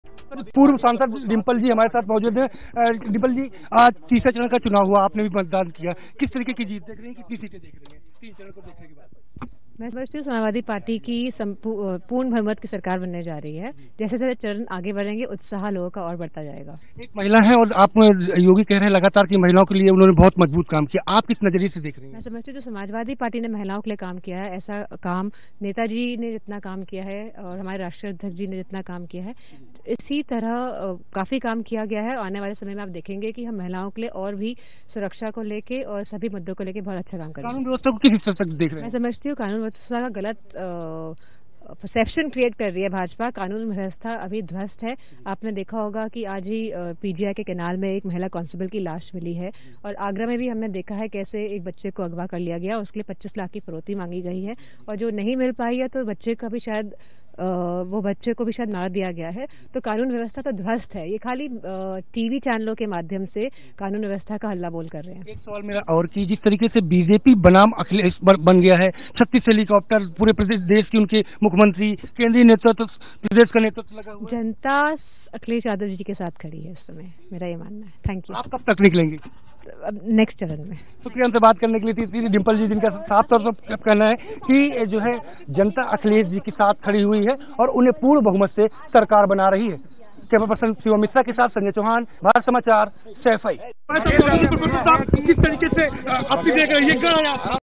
Bharat Samachar से बोली Dimple Yadav- BJP सरकार में कानून व्यवस्था ध्वस्त, 22 में बनेगी सपा की सरकार
Bharat Samachar से बोली DimpleYadav- BJP सरकार में कानून व्यवस्था ध्वस्त, 22 में बनेगी सपा की सरकार